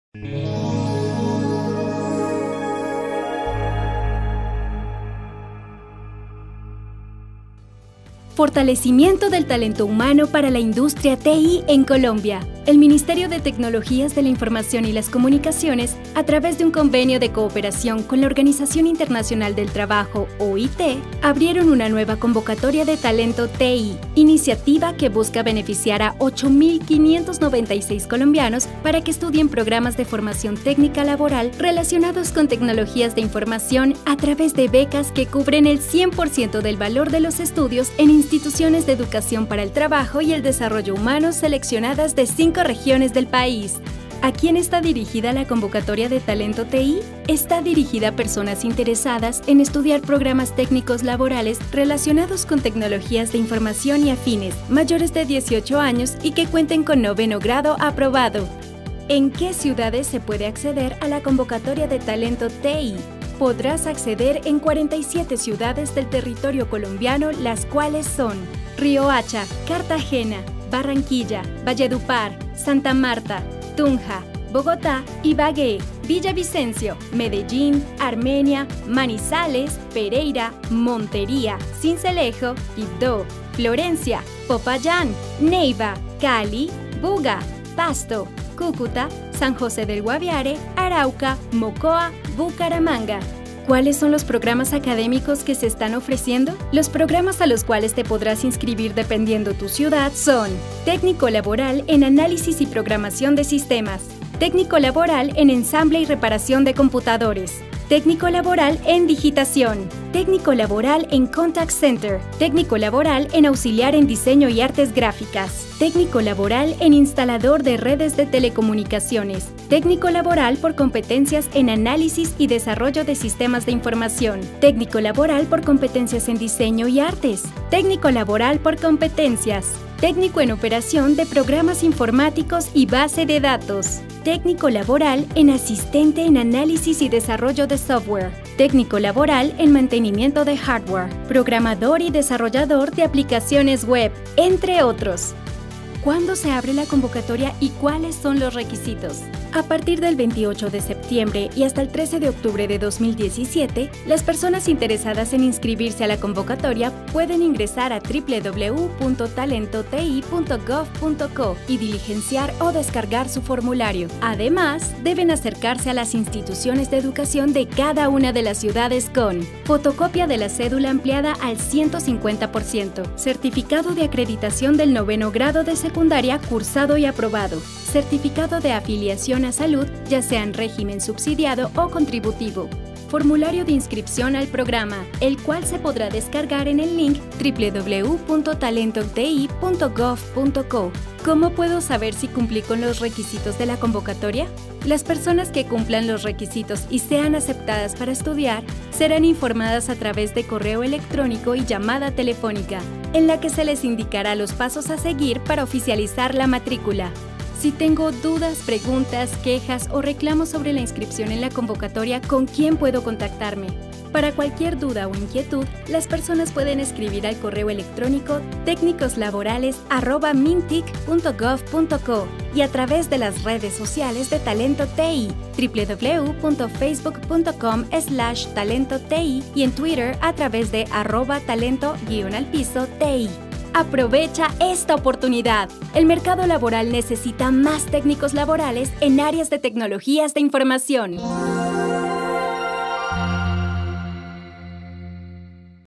Estudia GRATIS un técnico laboral en Tecnologías de la Infomación
En convenio entre Ministerio TIC y la OIT - se abrió la convocatoría de educación para el trabajo y desarrollo humano que beneficia a 8596 personas que quieran cursar un técnico laboral en TI.